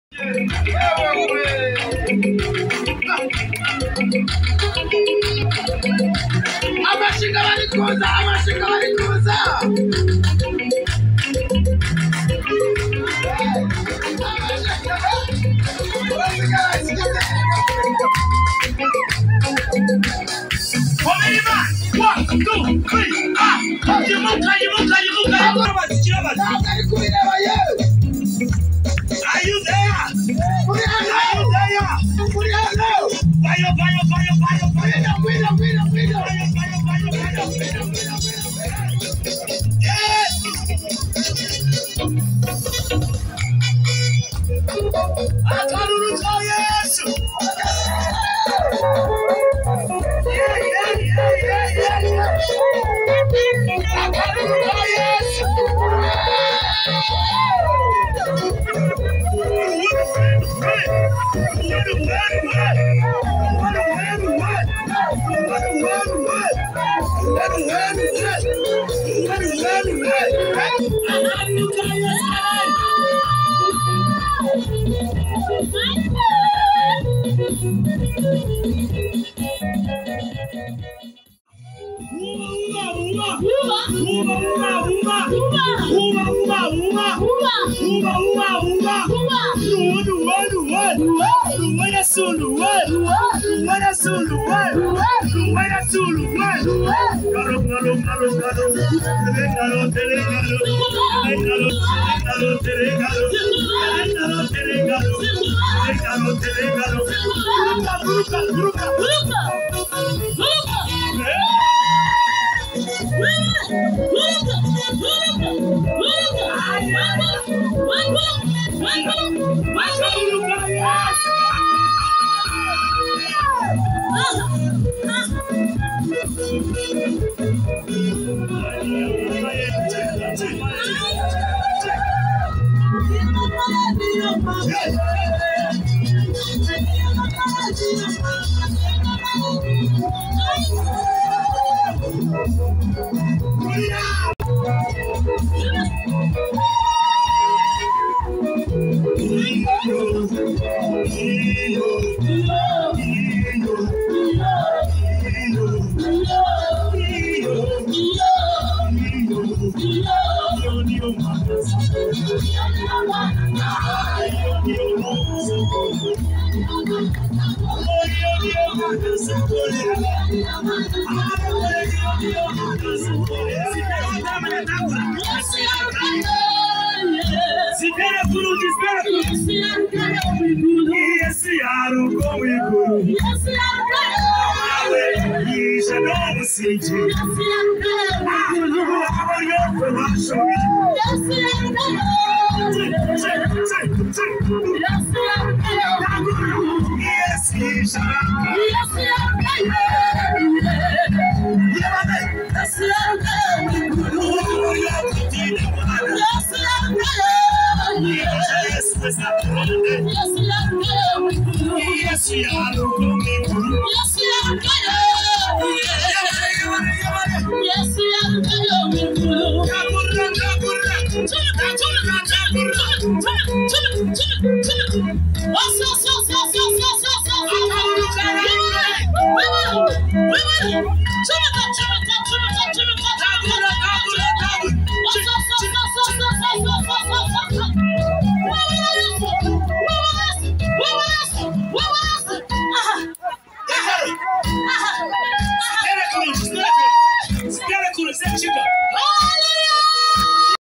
Pastor Grace & Hope Discovery choir saved alot of lives in kisoro
This was a mega worship night organized by Pastor Grace Mbabazi in kisoro, alot of people gave their lives to Jesus